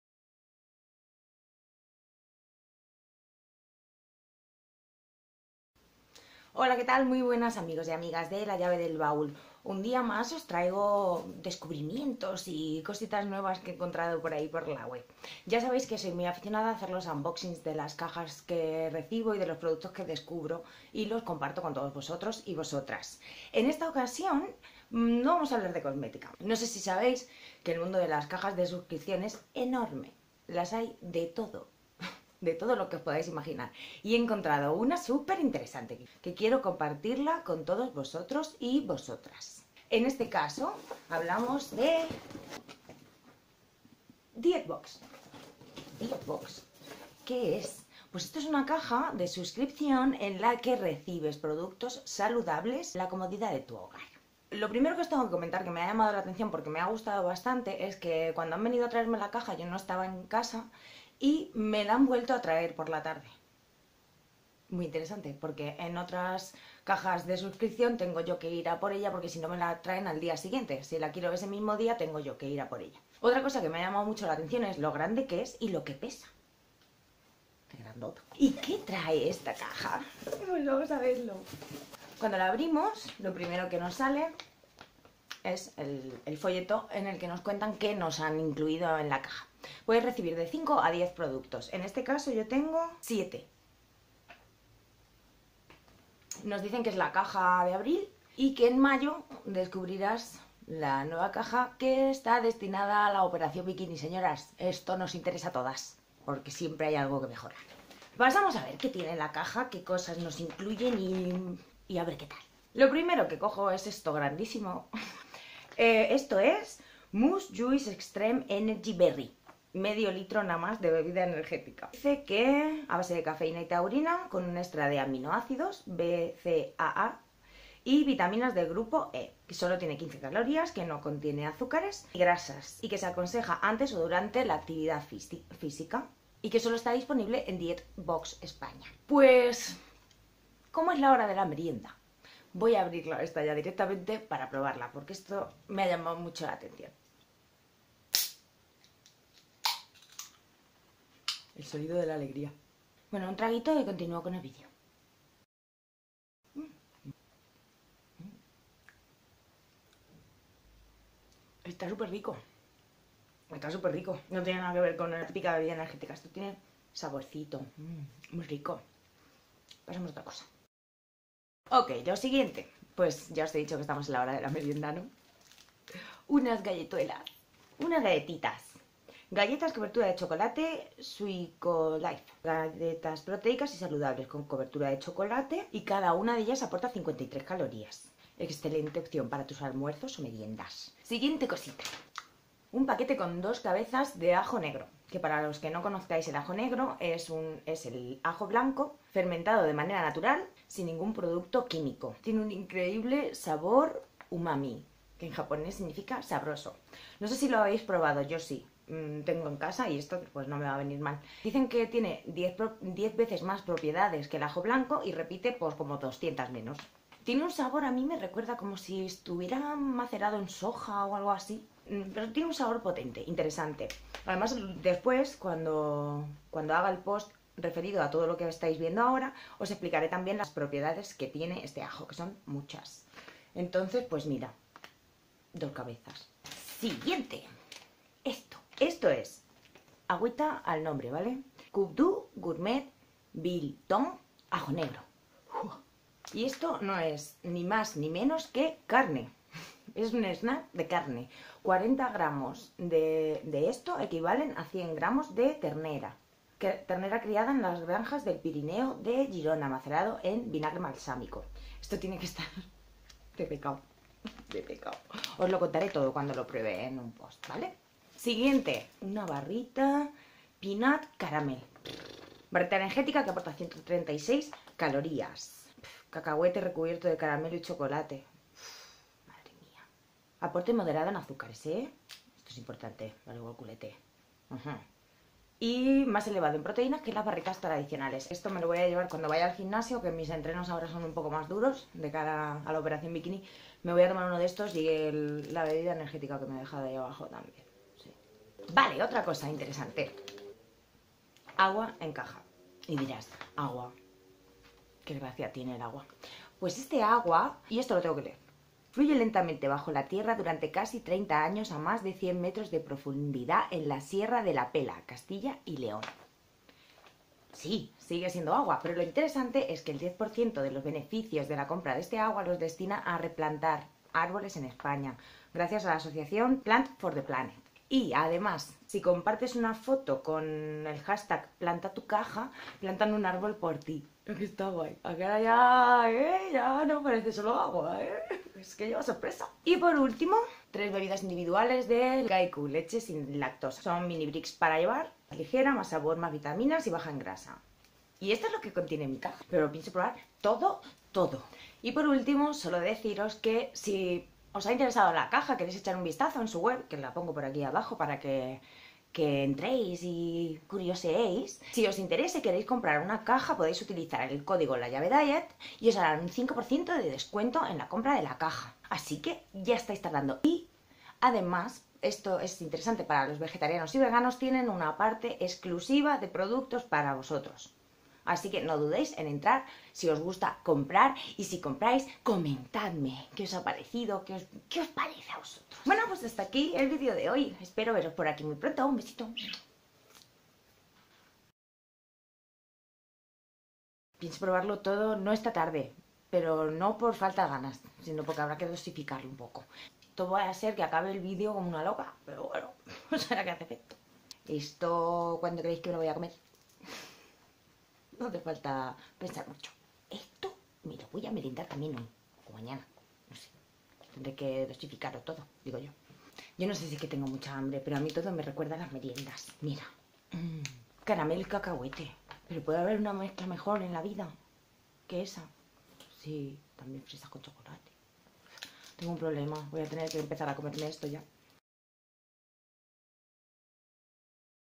Dietbox abril '17 🍪🍫. Unboxing
Unboxing de la caja Dietbox de abril de 2017, la caja saludable. Descubre qué contiene.